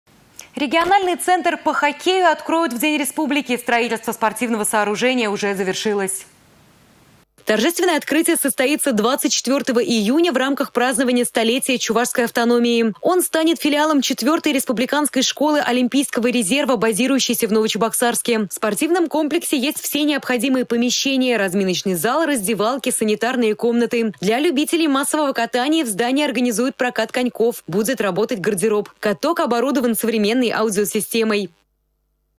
В День Республики откроют региональный центр по хоккею
Строительство спортивного сооружения уже завершилось.